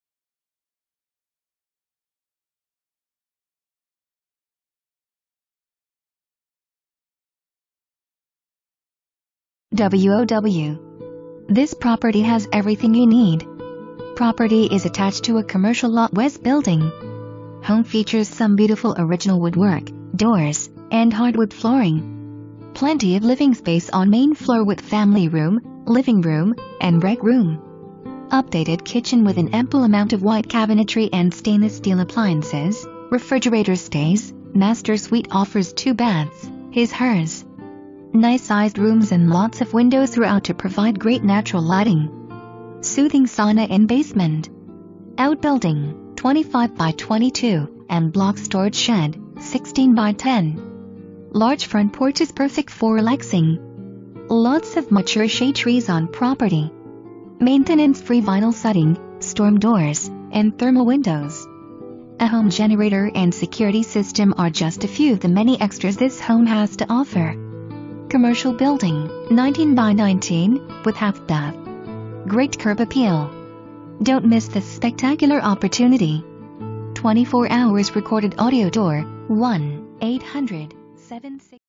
328 E Monroe Street Home
328 E Monroe Street
Buckner, MO 64016

WOW! This property has EVERYTHING YOU NEED! Property is attached to a Commercial Lot w/Building. Home features some beautiful original woodwork, doors, & hardwood flooring. Plenty of living space on main floor with family room, living room, & rec room.Updated kitchen with an ample amount of white cabinetry & stainless steel appliances. (Refrigerator stays) Master suite offers 2 baths (his/hers). Nice sized rooms & lots of windows thru-out to provide great natural lighting. Soothing sauna in basement. Outbuilding(25x22) & block storage shed (16x10). Large front porch is perfect forr relaxing. Lots of mature shade trees on property. Maintenance free vinyl siding, storm doors, & thermal windows. A home generator & security system are just a few of the many extras this home has to offer. Commercial building (19x19) with half bath. Great curb appeal. Don't miss this spectacular opportunity! 24 HR Recorded Audio Tour: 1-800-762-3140 #245.

Presented By:
Brad & Sonya Korn, Keller Williams Eastland Partners The Korn Team